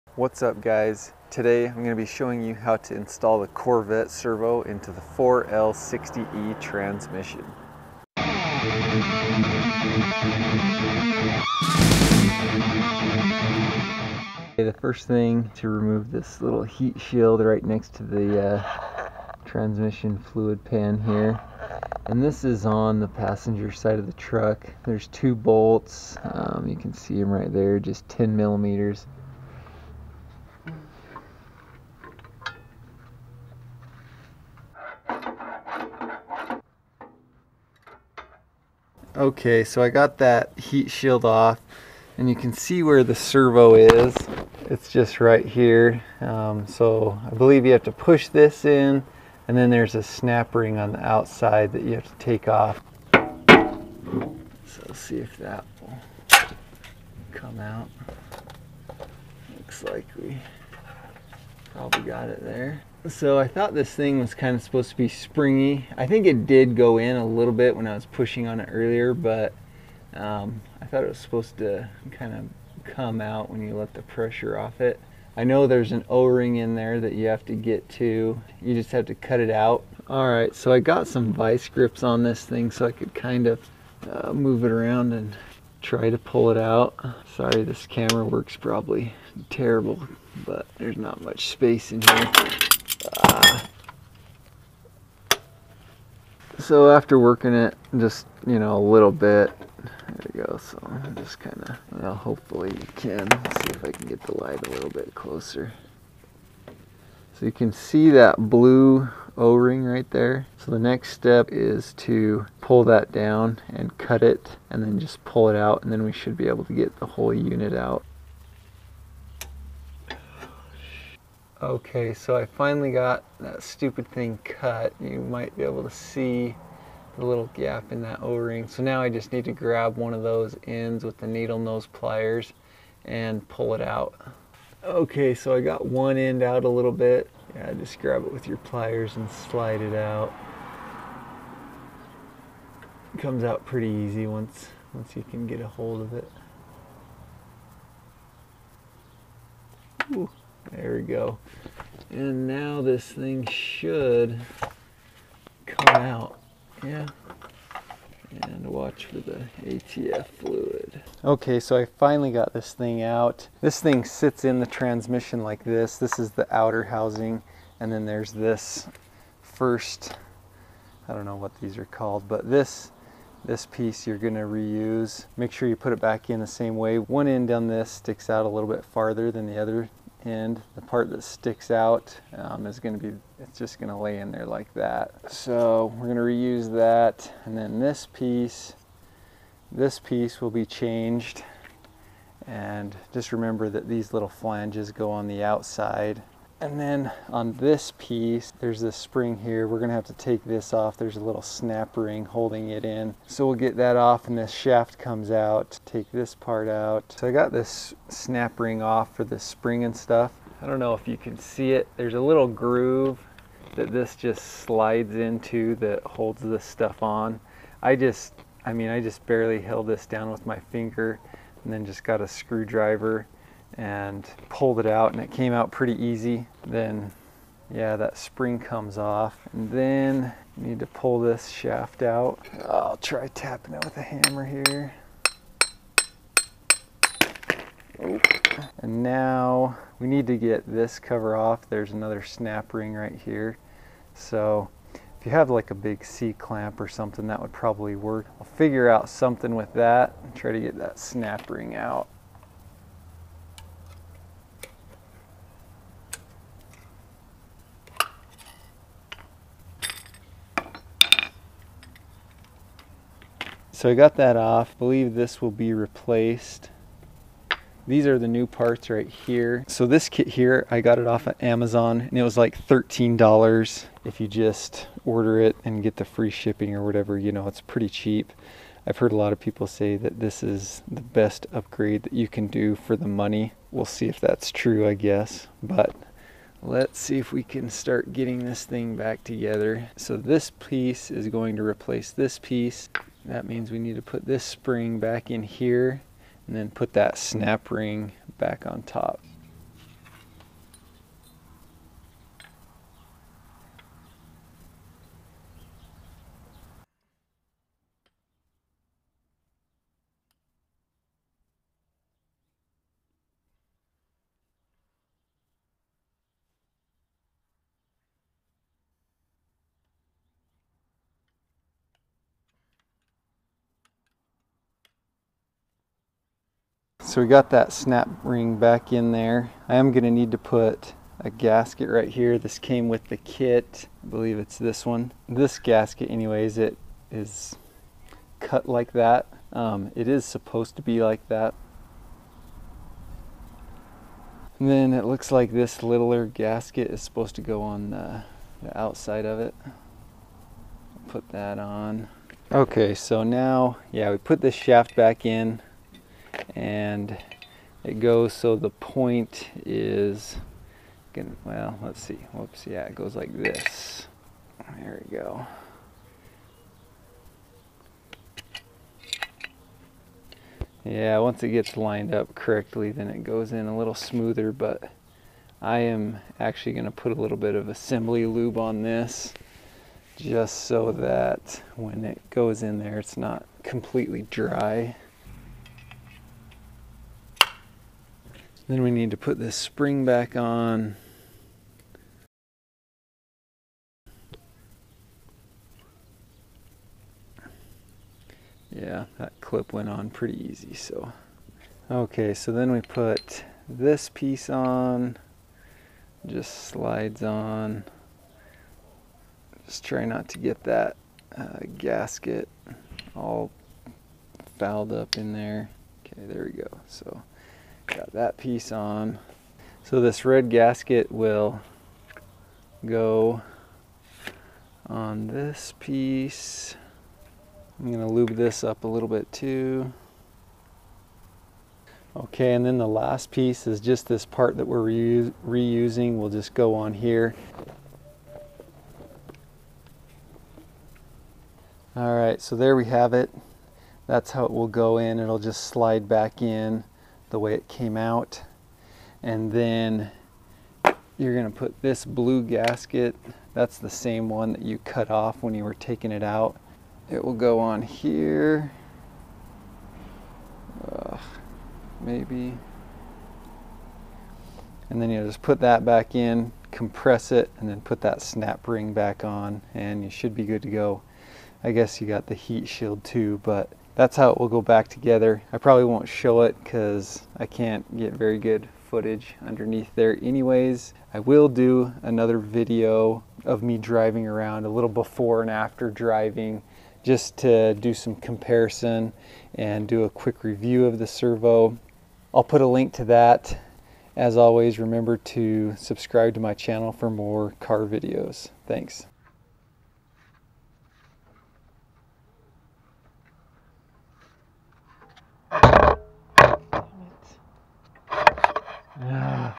4L60E Corvette Servo Install
Today I install the corvette servo into my truck.  the Corvette servo will work with any 4L60E transmission.  The part is cheap, the difficult part is getting the servo in and out.  It is in very tight quarters, but if I can do it so can you.